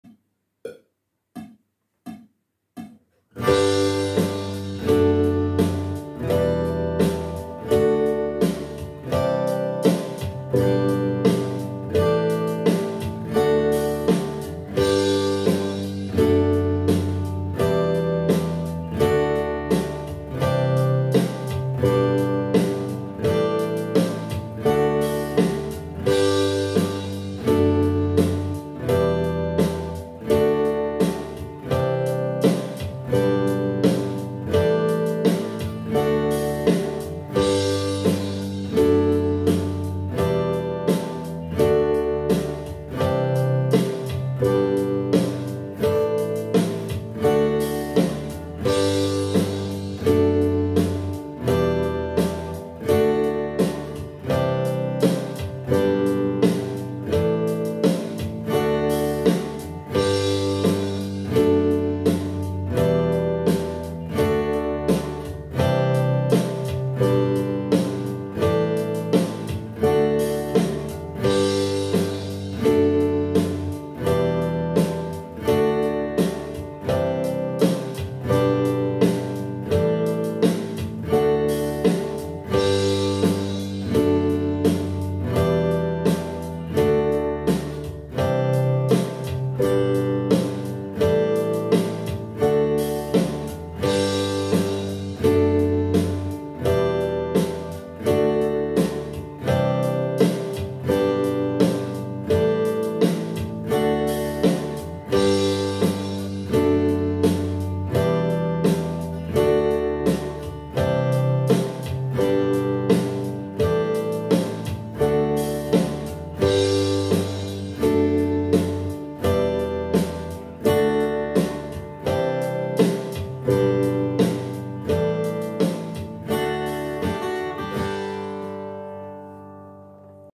Play Acoustic Guitar 1 - Lesson 3 "Slash Chords"
Practice log of TrueFire's Acoustic Learning Path. Play Acoustic Guitar 1 - Lesson 3

Guitar: Yamaha APX600
Amp: Yamaha THR30IIW